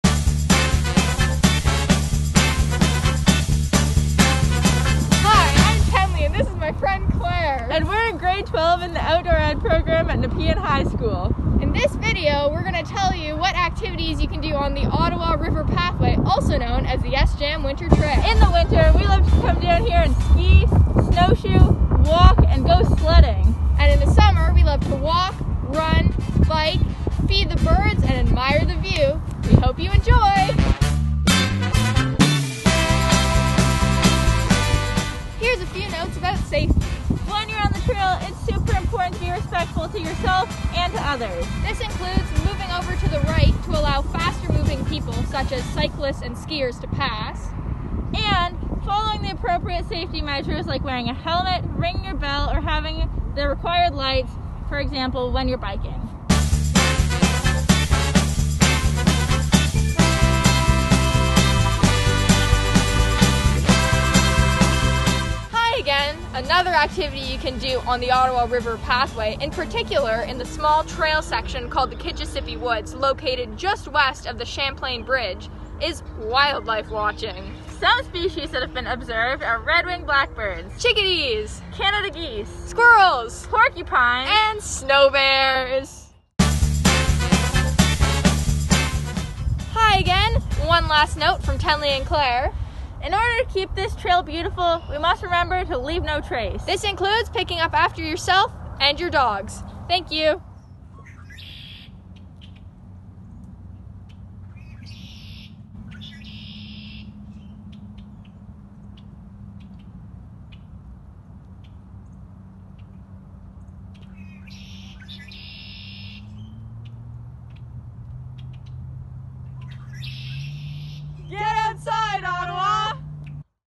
Your Guide to the Ottawa River Pathway
This video is made to advertise the Ottawa River Pathway and SJAM Winter Trail, and to encourage people to participate safely in outdoor recreational activities. Please comment if you have any questions, concerns, or feedback!